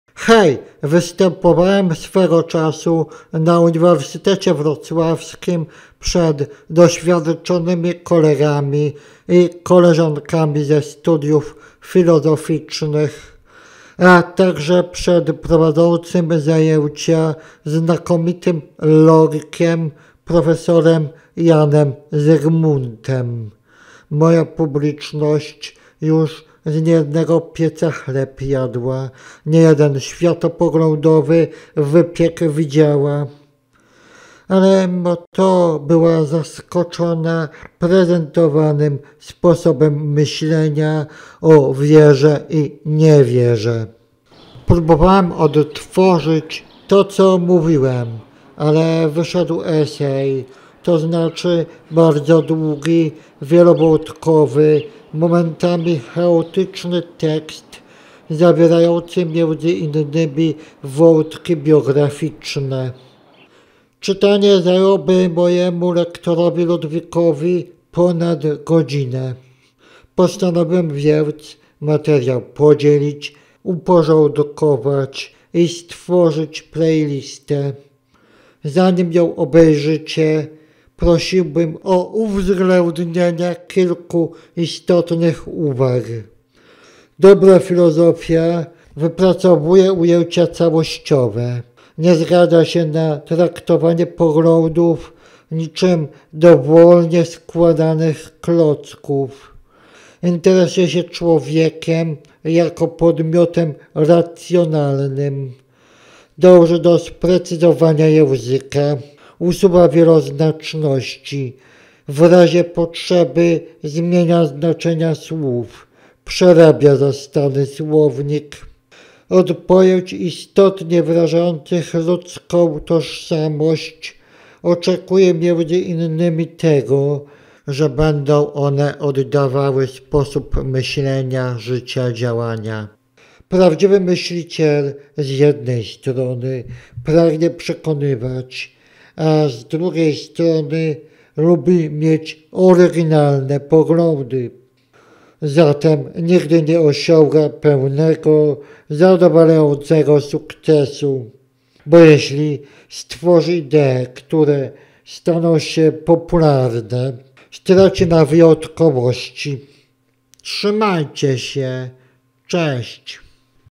ZAPROSZENIE DO OGLĄDANIA PLAYLISTY. O CZYM BĘDZIE...?
Przepraszam za to, że musieliście tak długo czekać na kolejny materiał Przedstawiam zaproszenie do oglądania playlisty. Filmy, które właśnie powstają, wyjaśniają, dlaczego nie wierzę w niewiarę. Materiały opierają się na długim eseju omawiającym spory światopoglądowe. Mam nadzieję, że opisem tego, o czym będzie..., zachęciłem do oglądania.